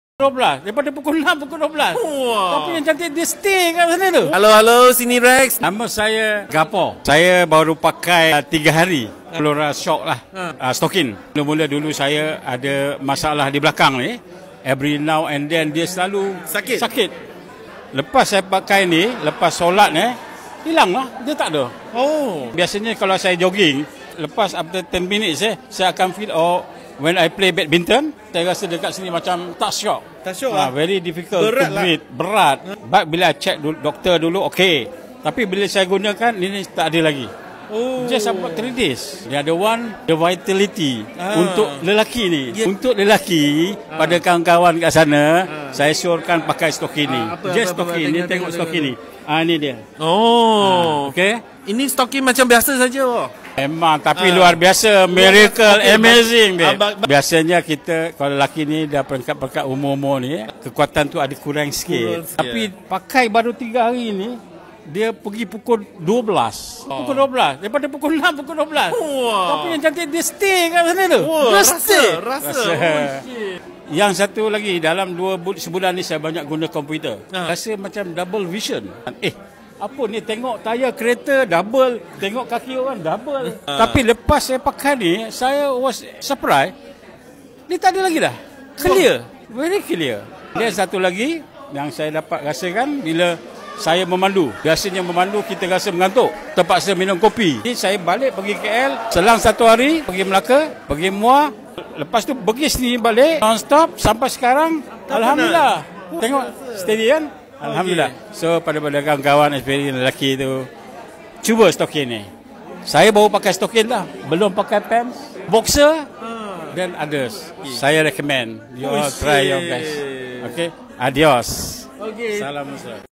WoW!!! Pakai Aulora Socks 3 hari je!!! 😱
Ini testimony yang begitu hebat sekali 😱 3 hari je! 3 hari je‼️
1. Bahu & leher x sakit
2. Dada x berat, nafas bertambah bagus
3. Vitality kekuatan lelaki 💪🏻💪🏻😱
4. Penglihatan mata bagus
5. Tidak mengantuk masa pandu

Jom dapatkan Aulora Socks !